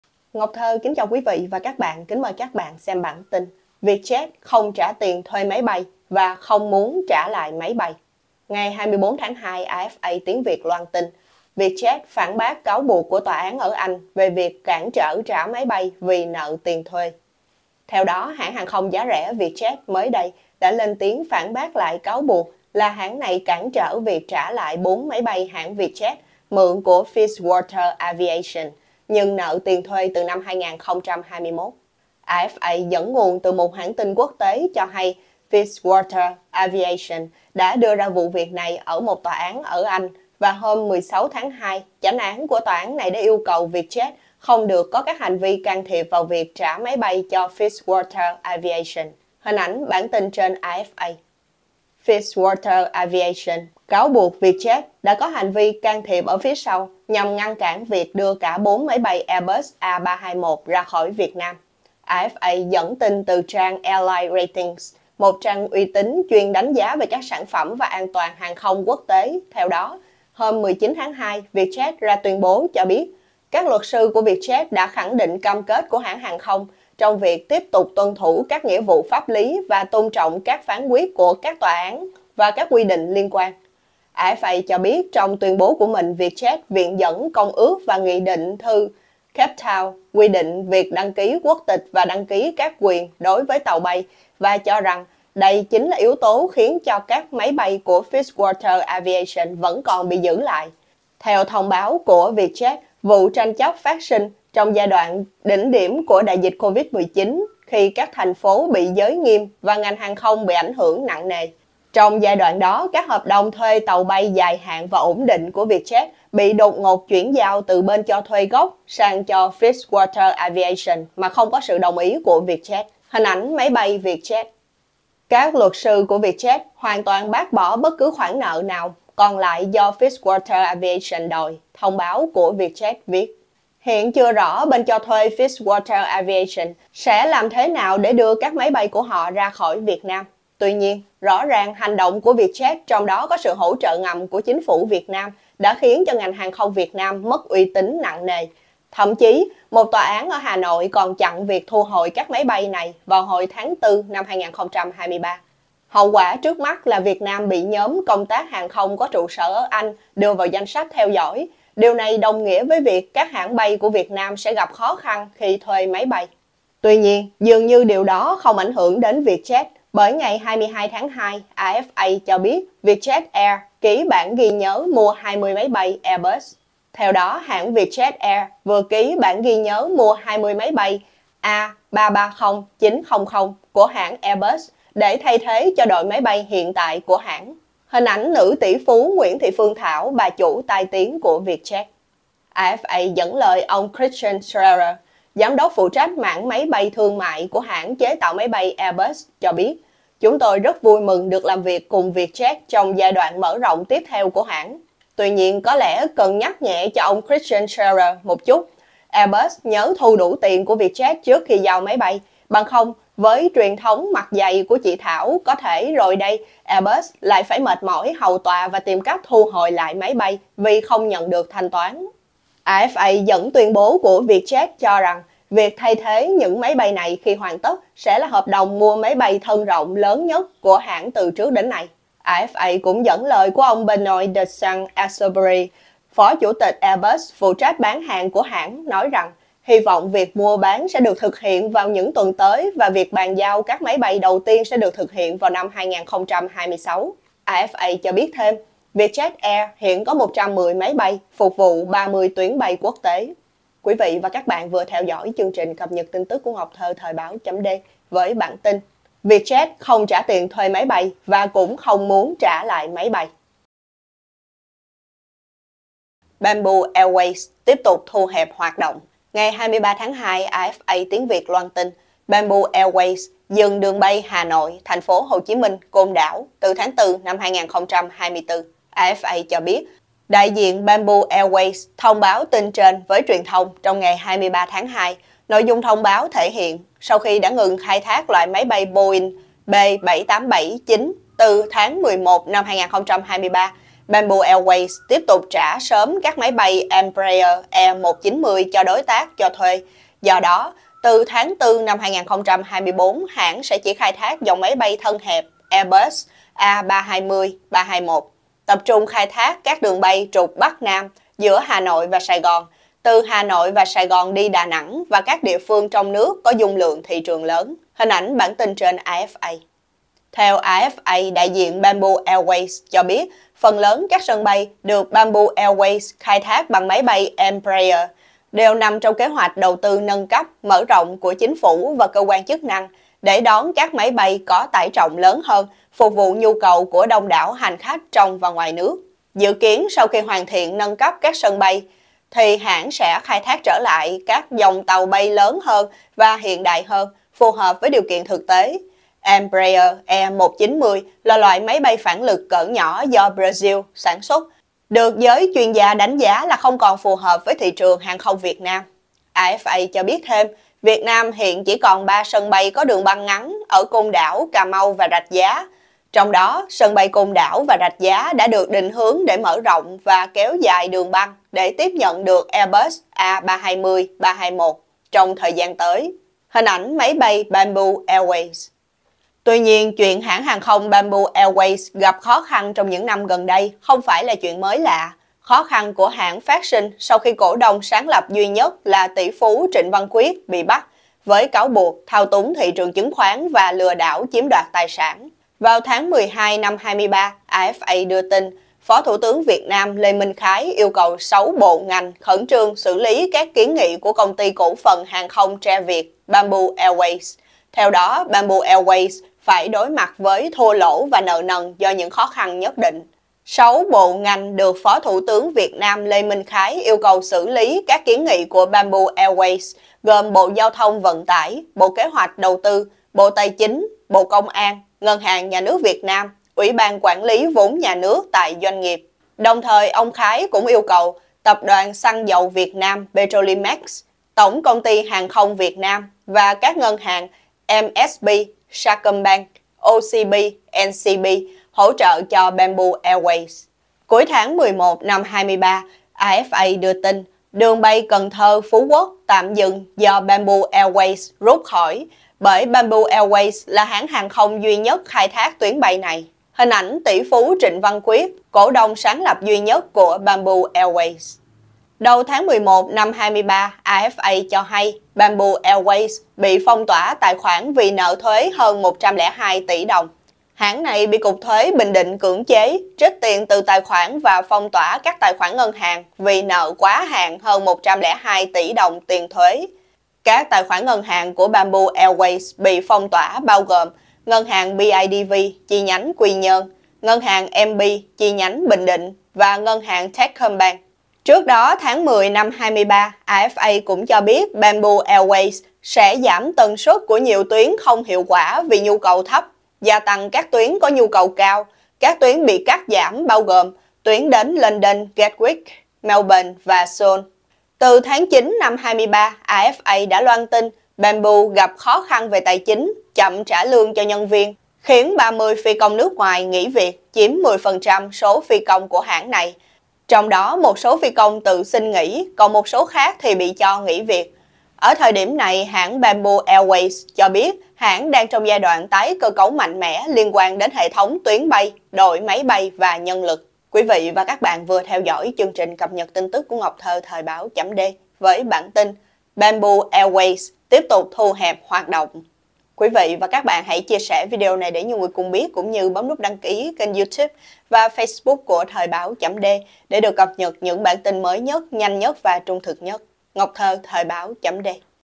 Vietjet không trả tiền thuê máy bay và cũng không muốn trả lại máy bay?
Hãng hàng không giá rẻ Vietjet mới đây đã lên tiếng phản bác lại cáo buộc là hãng này cản trở việc trả lại 4 máy bay hãng Vietjet mượn của FitzWalter Aviation, nhưng nợ tiền thuê từ năm 2021. Bất chấp việc hành vi cản trở này thể hiện quá rõ ràng trên thực tế, trong đó có sự tiếp tay của chính quyền Việt Nam.